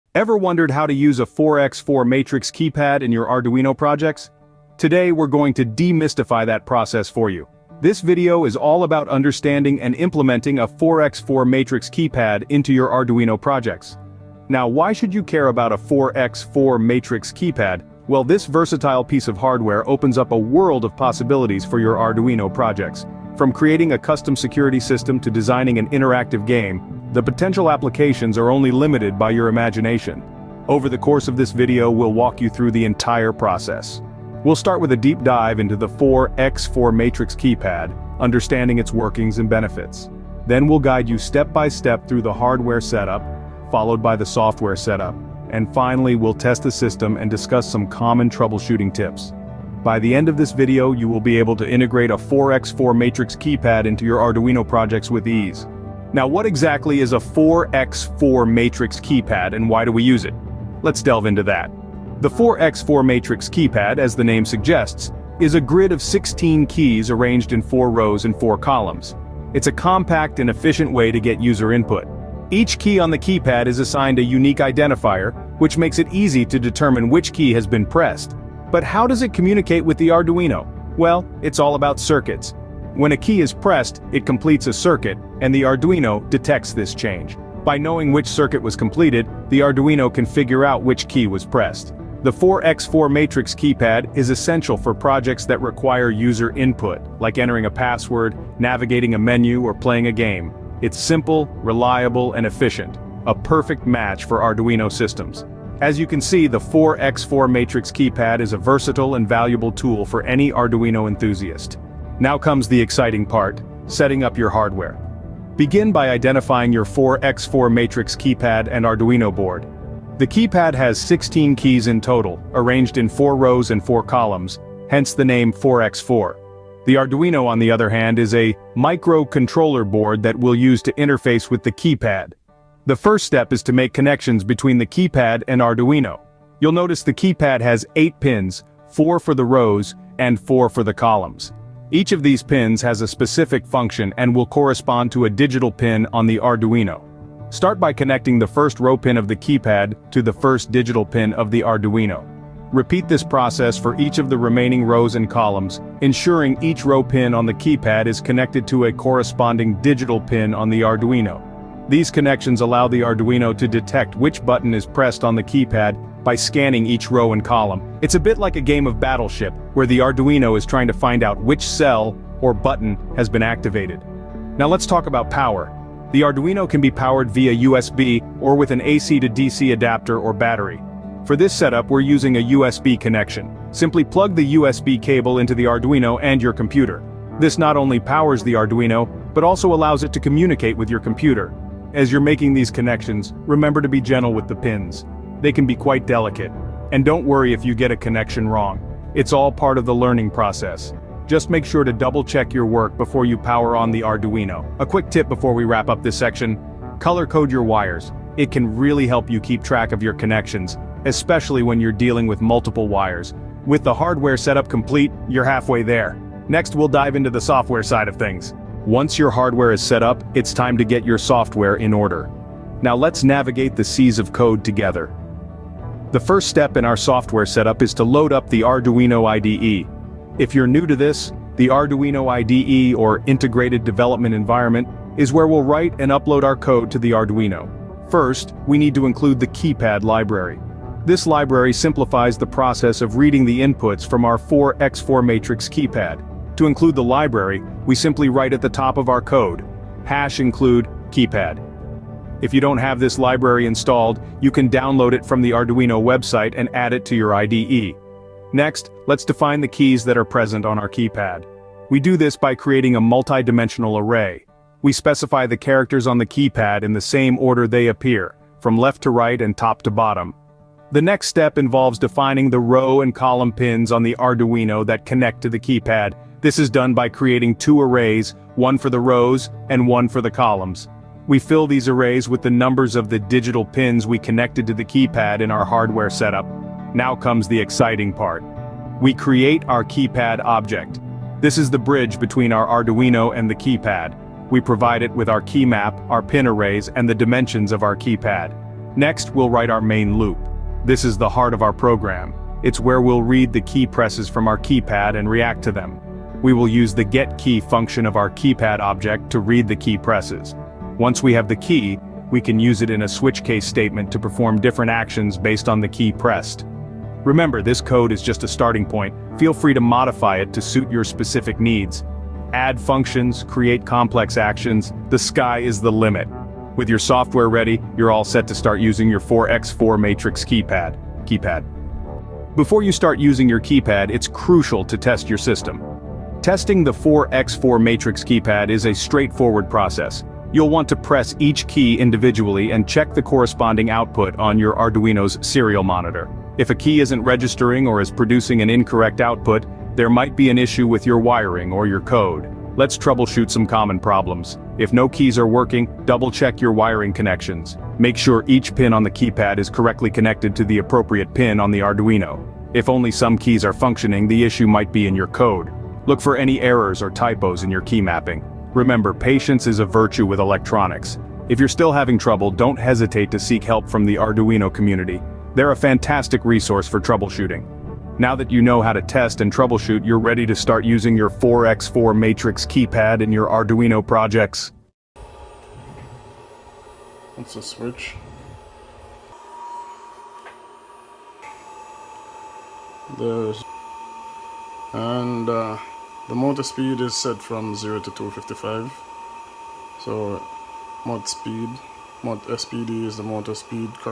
Mastering 4x4 Matrix Keypad - Part 3
A component by component buildup to the PID controller, this video shows the use of the 4x4 Matrix Keypad.